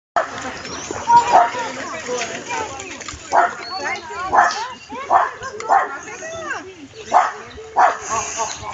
Aloha: best rope toy ever! (1-year-old golden retriever)
Aloha found a much better use for the boat binding rope.